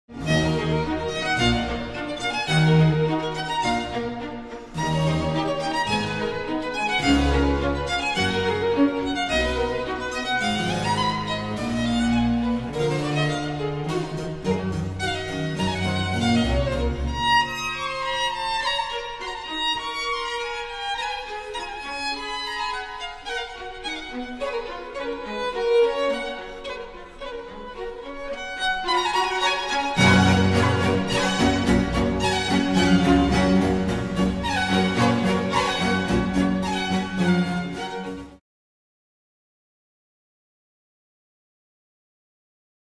Teaser - Le Hip Baroque Choc en tournage au Théâtre du Châtelet !
La préparation du spectacle Hip Baroque Choc 2021 se poursuit !

Le 18 décembre dernier, les élèves du groupe de danse Hip Hop du lycée professionnel Théophile Gautier ont pu réaliser le tournage de la seconde chorégraphie au Théâtre du Châtelet aux côtés de Julien Chauvin, des musiciens du Concert de la Loge et deux danseurs de la compagnie Käfig-CCN de Créteil.

Découvrez en vidéo ce moment enlevé par la musique de Vivaldi.